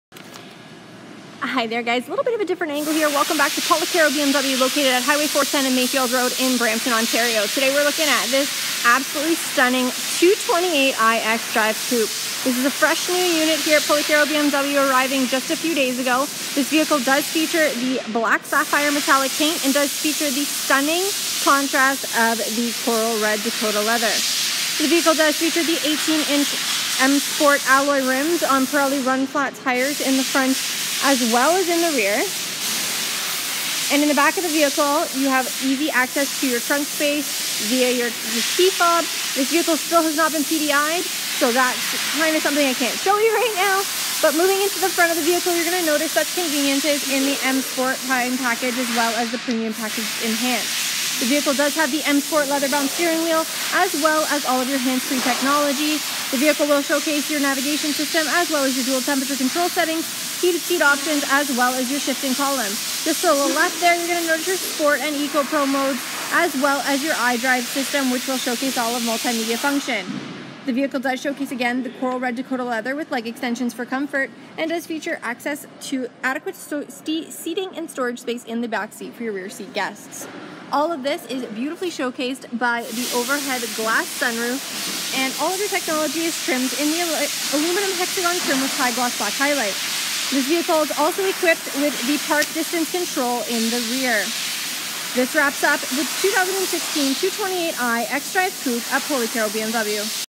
2016 BMW 228I XDRIVE COUPE AT POLICARO BMW
Black Sapphire Met with Coral Red Dkt Lthr
MSRP Options:  No Charge Scheduled Maintenance 4yr/80k;   Premium Package EnhancedPremium Package Enhanced
001 - Park Distance Control: 507 or 508
248 - Heated Steering Wheel
3AG - Rear View Camera
403 - Glass Sunroof
430 - Auto Dimming Exterior Mirrors
431 - Auto Dimming Mirror, Interior
459 - Electric Seats w/ Driver Memory
465 - Through Loading System
609 - BMW On-Board Navigation
6WA - Instrument Cluster, Ext.
;   Aluminum Hexagon Trim w/ High-Gloss Black Highlight;   Park Distance Control, Rear;   Black Sapphire Metallic;   Coral Red Dakota Leather